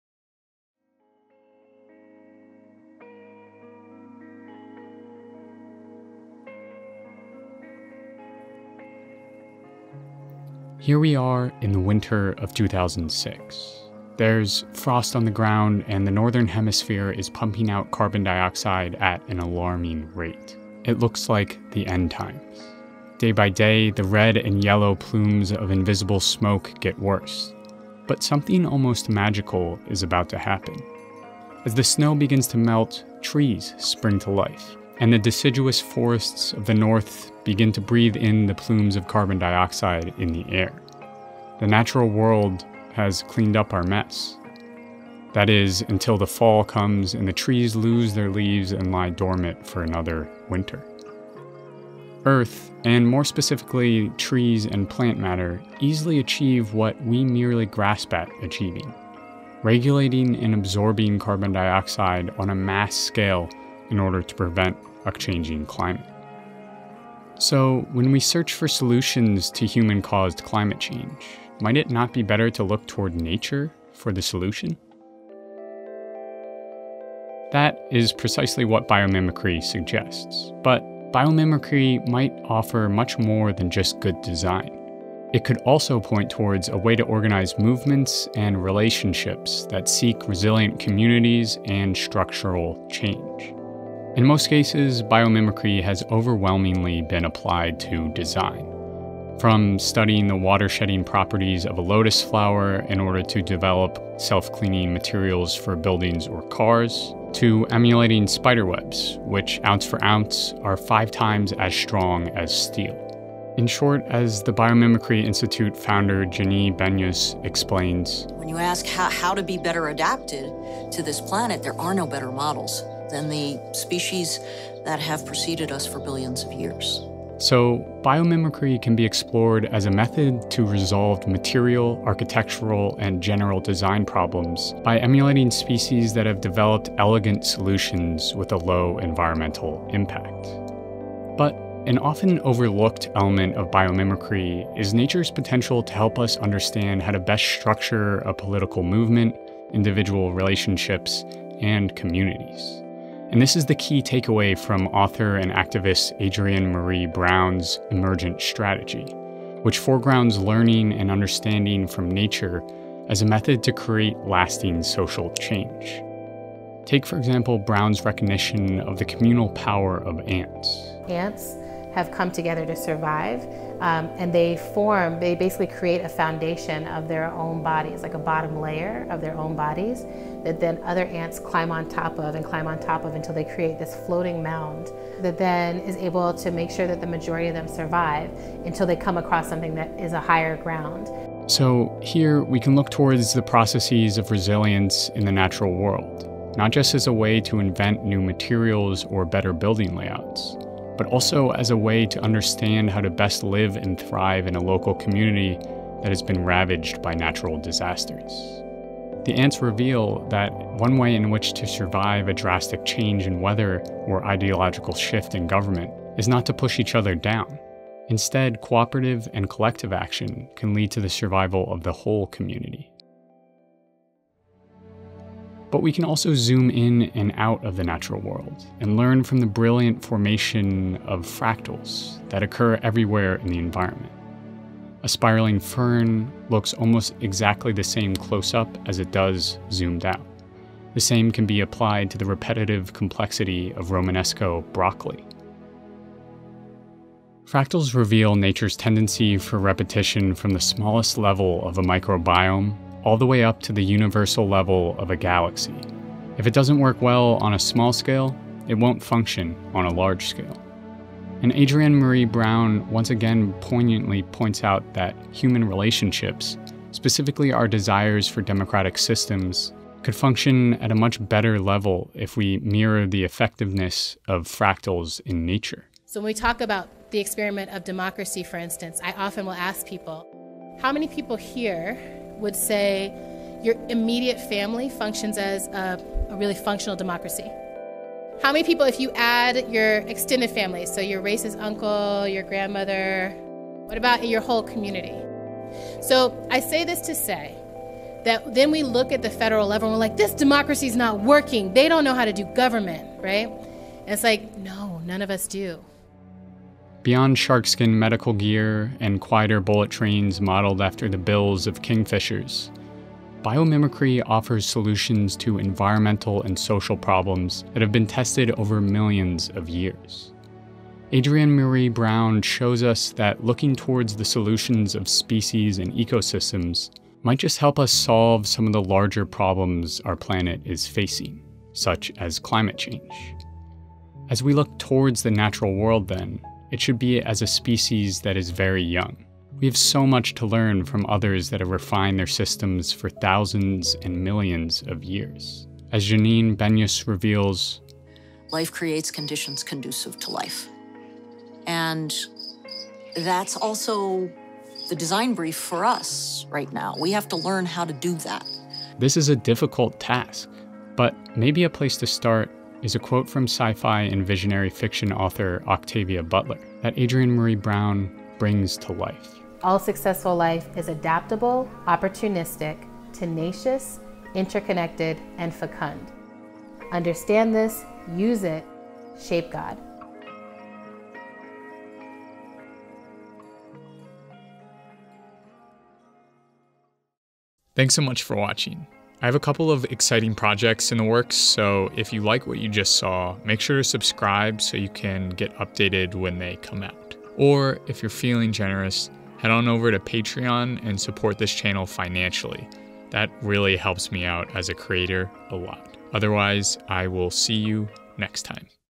Biomimicry is more than just good design.
Support OCC and get 20+ bonus, ad-free videos by signing up for Nebula

In this Our Changing Climate environmental video essay, I look at biomimicry. Specifically how biomimicry can not only lead to nature-inspired design for architecture and materials, but also for better relationships, activism, and communities. I draw upon adrienne maree brown's emergent strategy in order to show that nature and the environment can show us how to best navigate a complicated social world.

Resources:
1. Emergent Strategies by adrienne maree brown